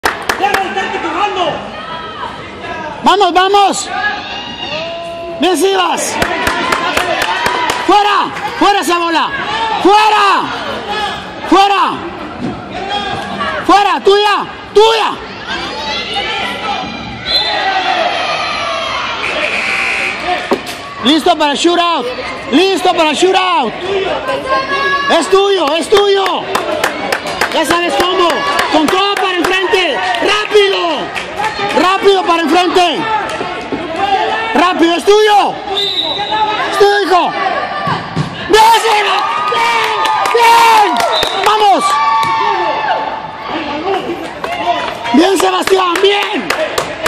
Amazing 9 years old Goalkeeper - Sebastian Lutin - Great Saves
Playing for Fire F.C. Two great saves, amazing penalty shoot out save, with coach Gerardo Reyna. Keep it up!!